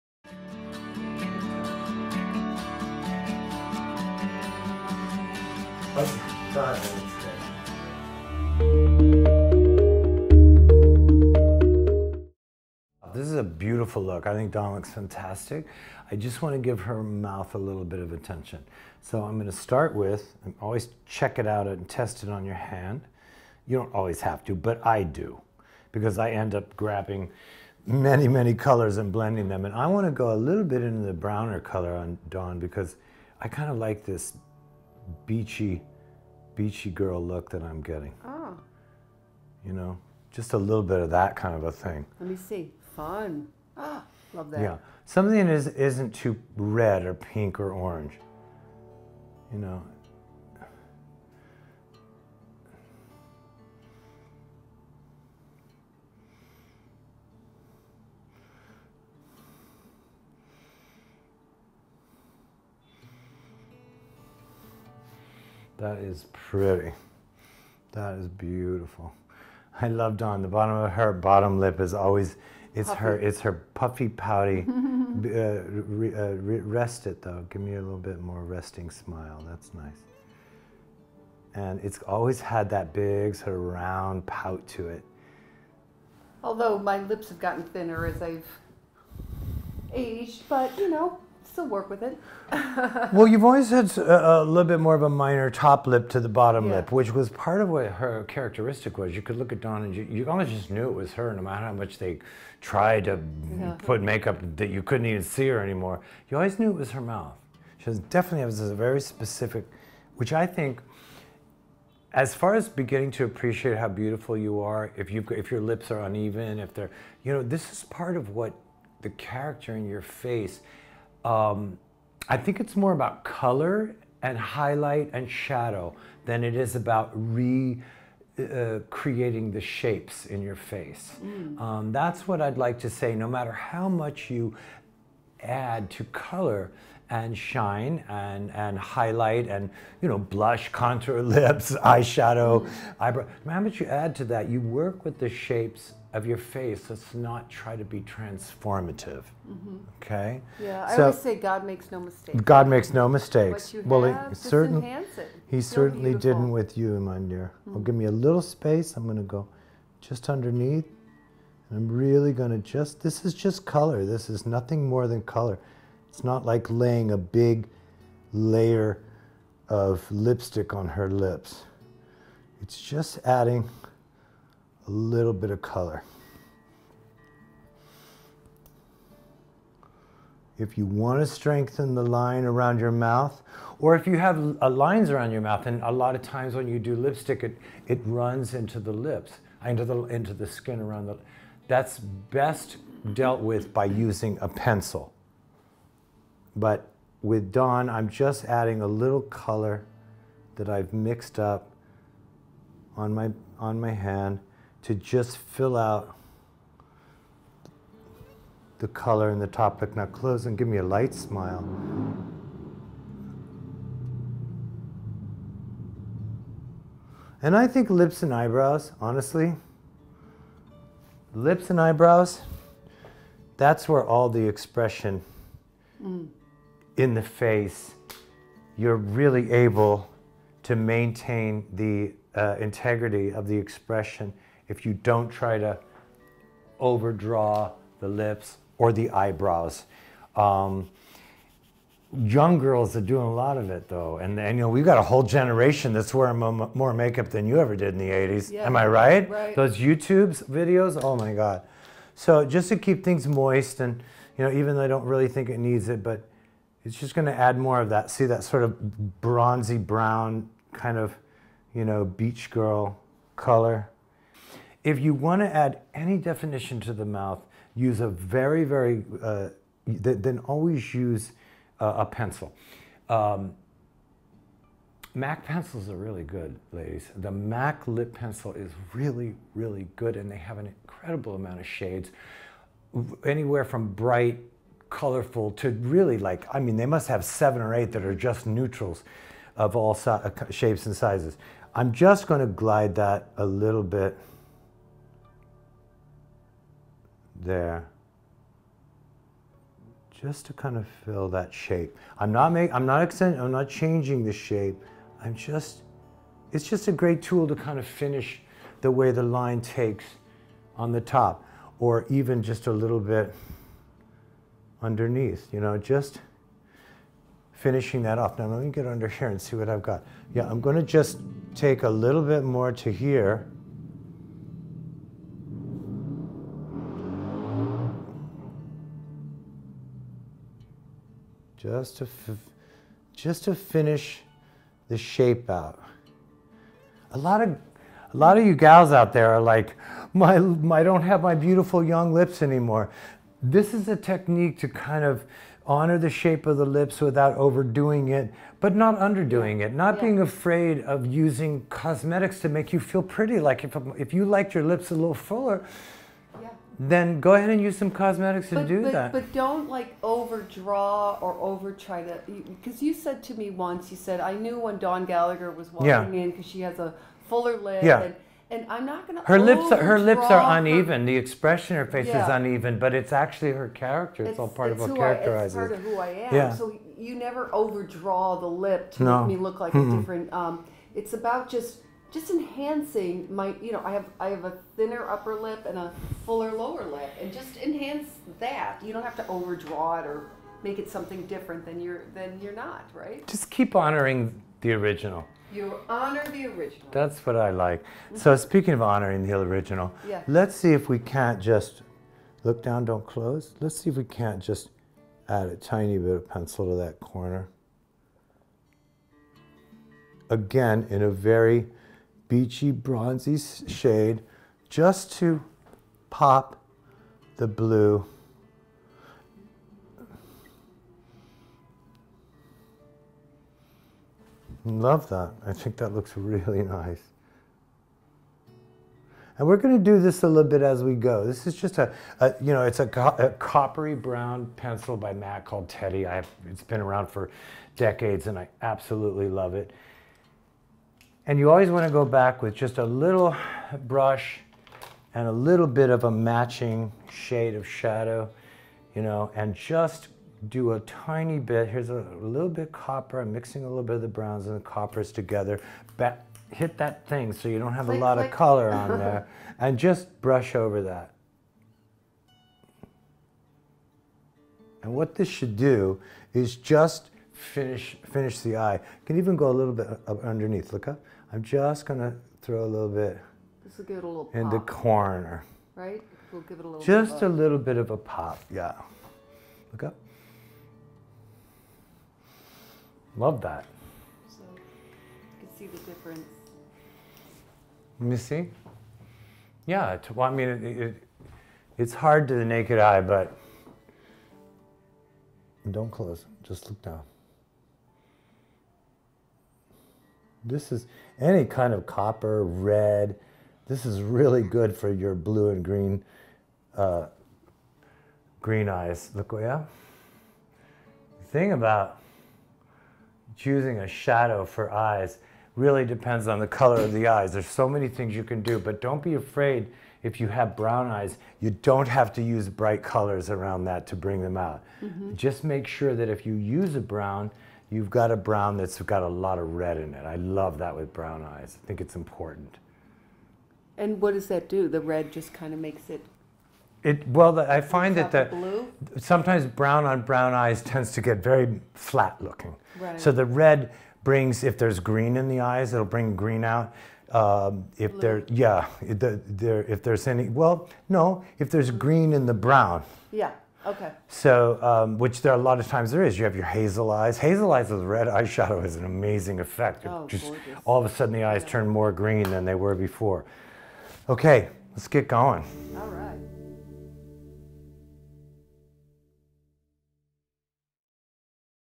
Makeup for Women Over 50 | Dawn and Joseph
In this Part 4 video of Makeup for Women over 50, Dawn Gallagher and Joseph Boggess give you behind the scene makeup tips to make you feel beautiful. See PART ONE, TWO and THREE getting the skin ready. Our purpose to make women over a certain age feel gorgeous and it's not game over, it's game on!

Makeup list:
Kiehl's Since 1851 Calendula Herbal Extract Alcohol Free Toner

Smart Cover Perfect Touch Camouflage Creme Concealer- Creamy, Blendable, Buildable, Full Coverage Concealer, Cover Imperfections, Dark Eye Circles, Age Spots- Neutral/Dark

Ready To Wear’s® BROW SKETCHER - BRUSH ON BROW SHAPER

Starry Long Lasting Waterproof Eyeliner Gel with Brush Choclate Brown by STARRY

To contact Makeup artist Joseph Boggess please go and follow him on instagram @jospehboggess2197.

About this video: In this video Dawn Gallagher and Joseph Boggess give you Youthful Day Makeup Tutorial for Mature Skin! For all women but especially over 40, 50, 60 and beyond. Beauty is ageless!